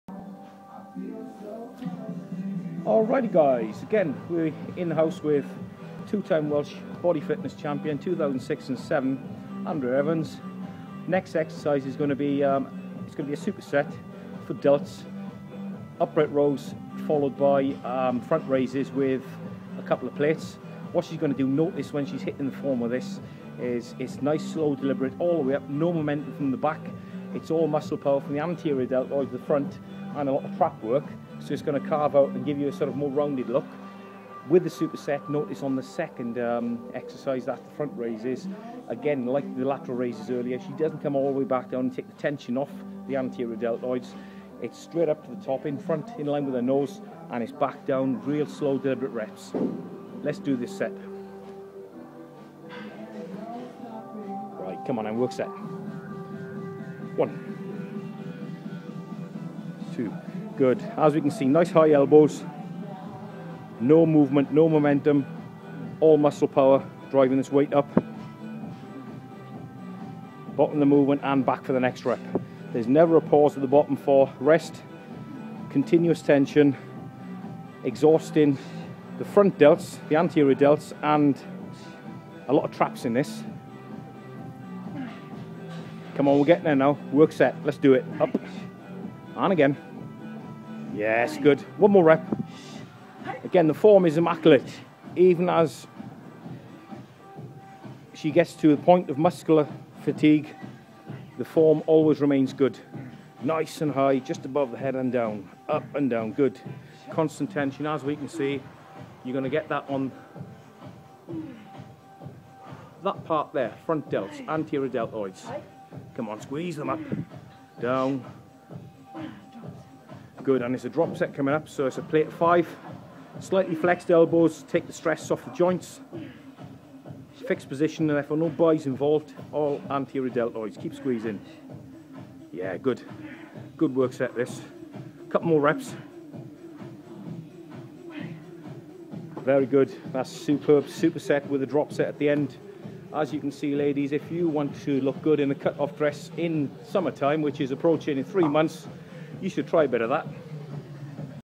Female Fitness: Andrea Evans 2 time ukbff Welsh bodyfitness Champion.
2 x welsh ukbff champion and EPF UK figure Champion shows you girls how to train to look good in Summer clothing.